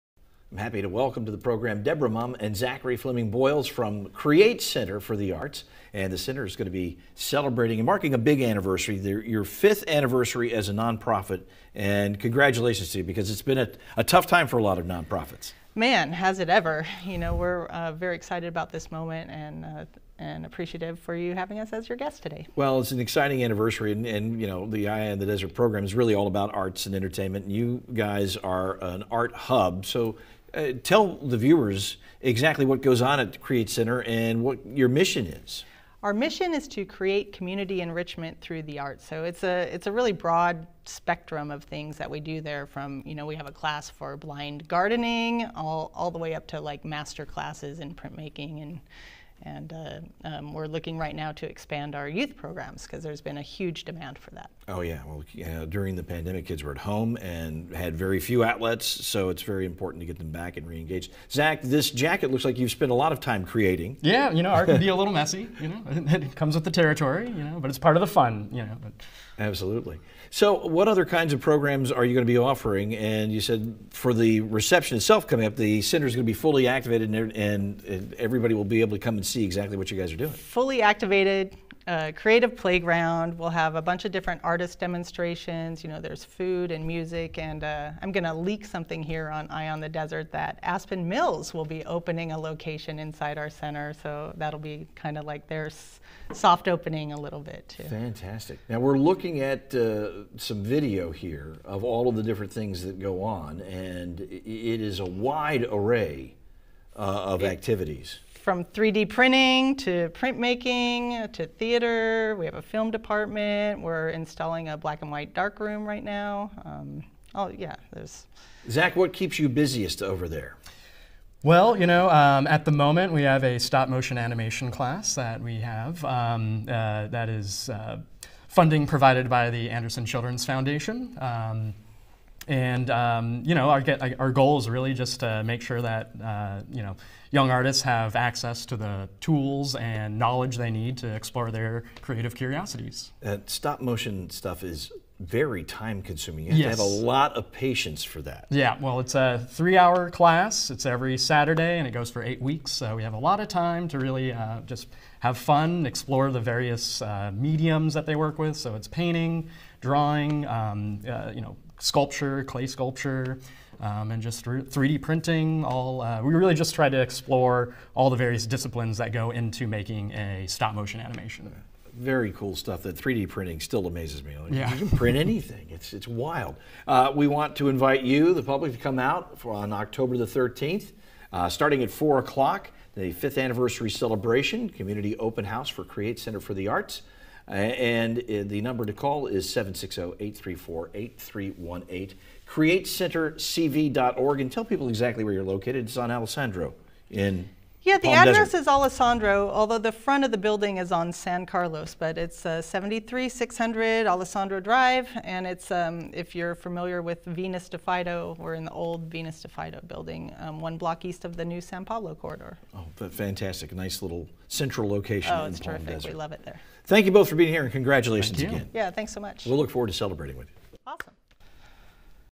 The Create Center of the Arts discusses how they are celebrating their 5th year as a ...
The Create Center of the Arts discusses how they are celebrating their 5th year as a non-profit